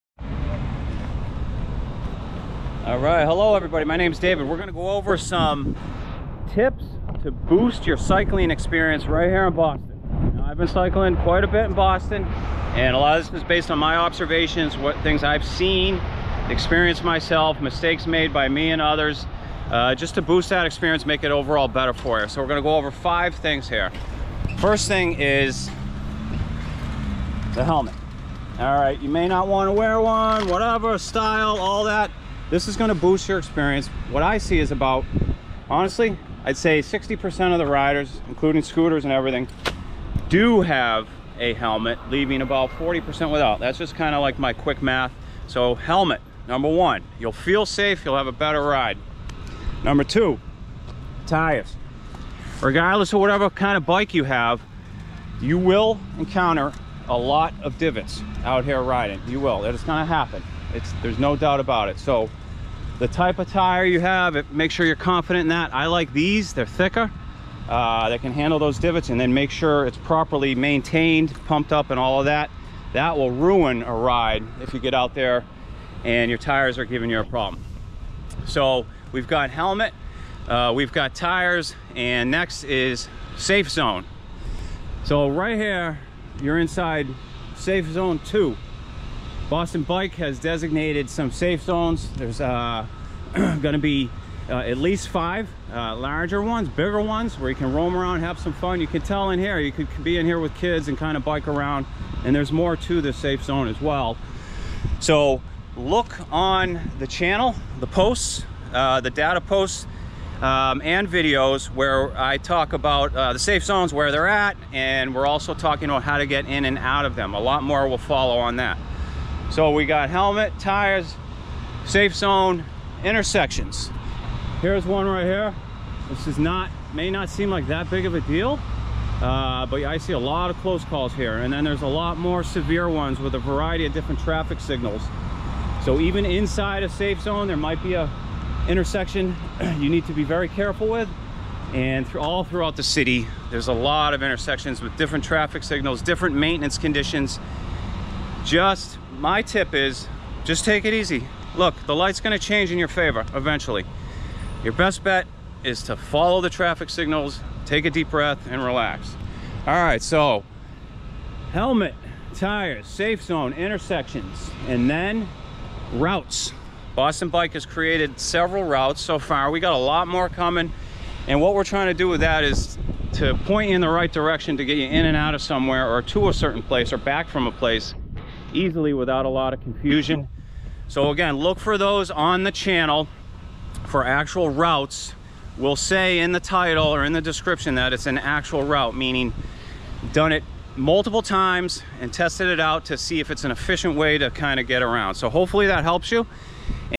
Maximize Your Ride: 5 Exclusive Boston Cycling Tips
Ready to boost your cycling experience in Boston? In this video, we're sharing insider tips and tricks for cycling in the city. Whether you're a beginner or a seasoned rider, these tips will help you navigate Boston's streets with confidence.

5 tips here from someone that cycles a lot in Boston, Brookline, and Cambridge.  Justsome observations . . . . . enjoy it!

Many other videos to help with the safe zones, routes, other tips, etc.

Cycling Lanes and Paths in Boston, Massachusetts

Boston provides an outstanding network of bike lanes, off-road paths, and bike-friendly infrastructure. The city is continuously investing and upgrading to provide more accessible and safer spaces for cyclists.

  The Paul Dudley White Bike Path: A 17-mile loop along both sides of the Charles River, offering great views of the Boston skyline. It features areas for resting and picnic spots.

The Emerald Necklace: This is a chain of parks and pathways that starts at the Boston Common downtown and heads southwest for about seven miles until it reaches Franklin Park.

 The Minuteman Bikeway: A 10-mile rail-trail that stretches from Alewife Station in Cambridge to Bedford. It's one of the most popular bike paths in the Boston area, known for its scenic views.

 The Southwest Corridor Bike Path: This is a 4.7-mile path that extends from the Back Bay to Forest Hills. It passes through several neighborhoods, providing a convenient commuting route.

 Neponset River Greenway: A nearly 9-mile path that runs along the Neponset River from Dorchester to Hyde Park, crossing diverse neighborhoods, marshes, and parks.

In addition to these paths, Boston is also home to Hubway, a bike-sharing program with over 100 stations and 1,000 bikes available. Furthermore, there are several ongoing projects aimed at expanding the cycling infrastructure to encourage safe and sustainable transportation.

It's worth mentioning that the City of Boston has invested significantly in promoting cycling. As part of its 'Vision Zero' safety initiative, Boston is working to eliminate fatal and serious traffic crashes by 2030, forming part of its overall plan to create safer roads for cyclists.